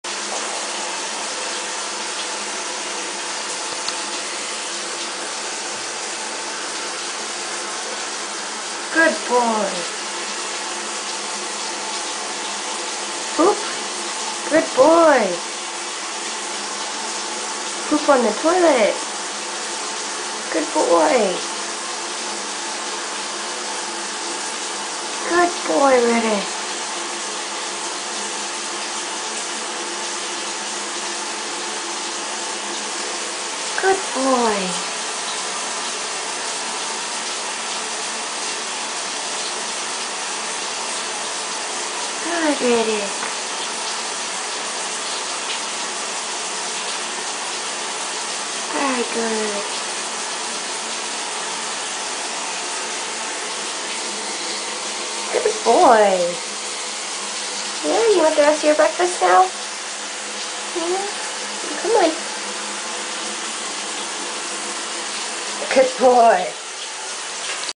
Riddik the iguana uses the toilet
It has taken a total of about 2 months to get him to this point in his potty training. He started out by pooping in his fountain in his water dish. Then, I moved the fountain to a larger water dish on the floor of his enclosure to show him that was the proper place to poop. He followed the fountain to the larger dish and established a routine of going to the floor of his enclosure to poop on the fountain in the large water dish. I eventually removed the fountain and he continued to seek out the large water dish to do his business. Then, When I went on holiday for Christmas, I was waking up later, and able to bring him to the shower when he was ready to do his business. after about 4 days of this, he learned that the sound of running water means it is time to poop. He started trying to poop on the counter where he eats breakfast because he could hear his bath water running. I had to move him over the sink  because he was already about to go, and would not make it across the bathroom to the tub. He continued to try to poop next to his food dish because of the water sounds for the next 4 or 5 days. Then, I started picking up on his signs of being ready to poop and was able to move him over the toilet to do it a couple of days ago. The next day, I held him over the toilet before he showed signs of needing to poop, and he did it anyway. Today I layed towels across the seat for him to balance on by himself, ran the bath water, and put him down. This is his first successful poop on the toilet all by himself.